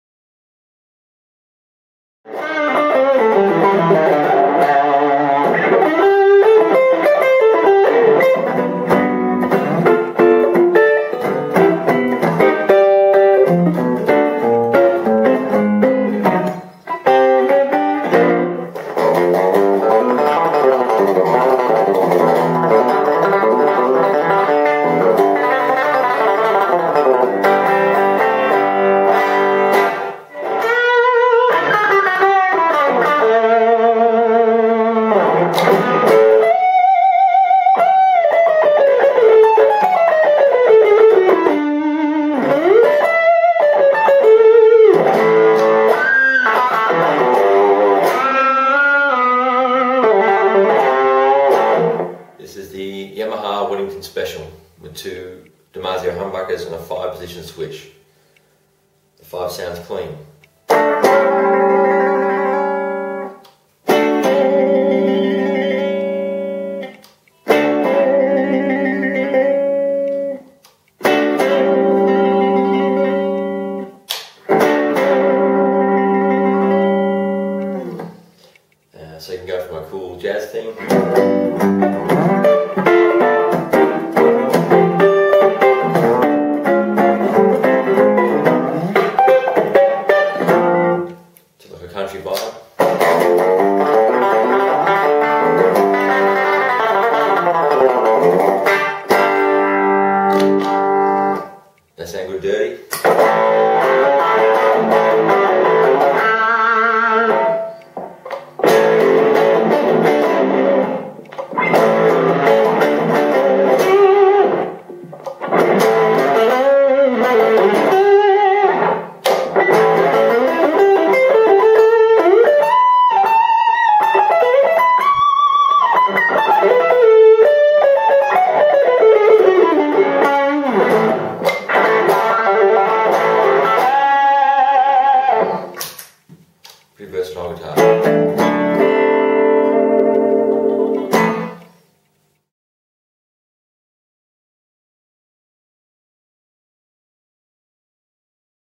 1998 Yamaha Weddinton Special DEMO
Jimi Hocking demonstrates a 1998 Yamaha Weddington Special, through a VOX AC-4 and a Screaming Tubes pedal.